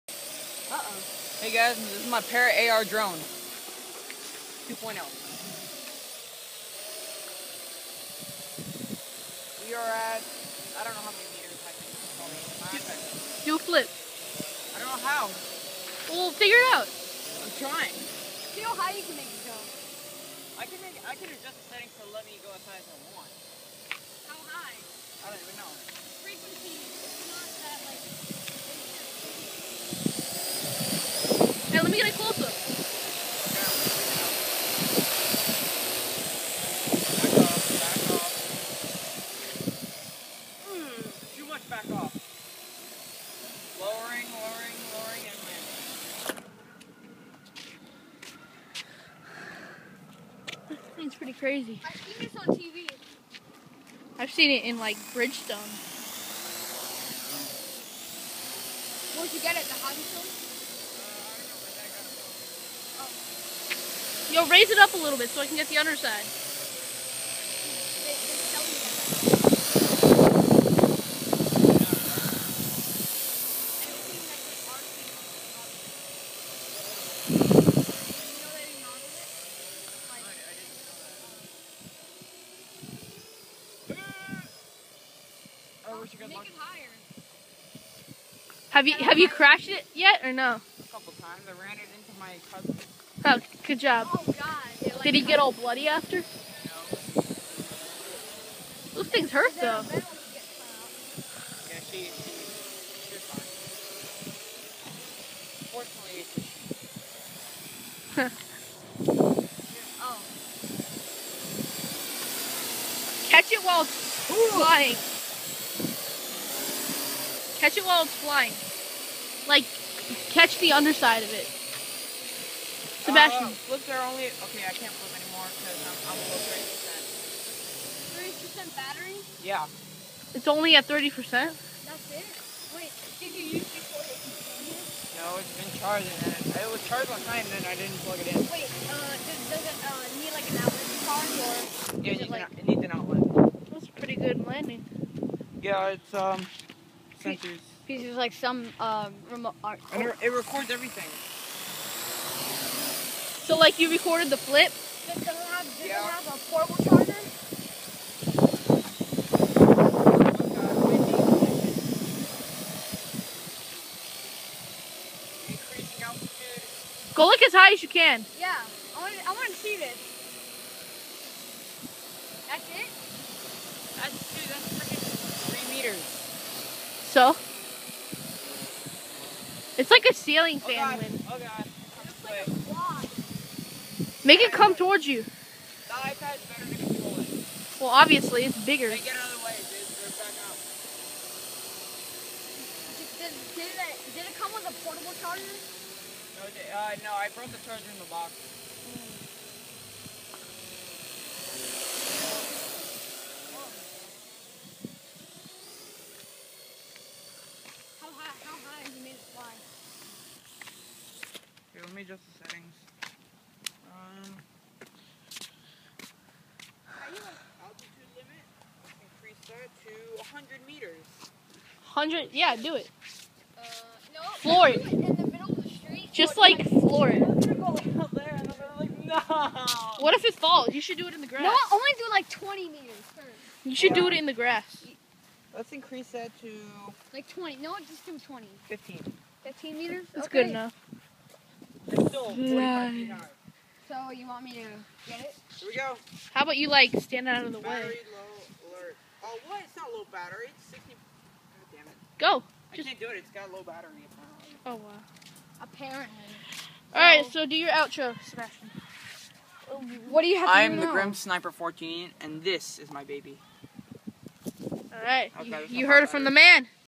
Parrot AR Drone 2.0
My friend wanted to show me his new gadget so I took a vid of it. ENJOY!!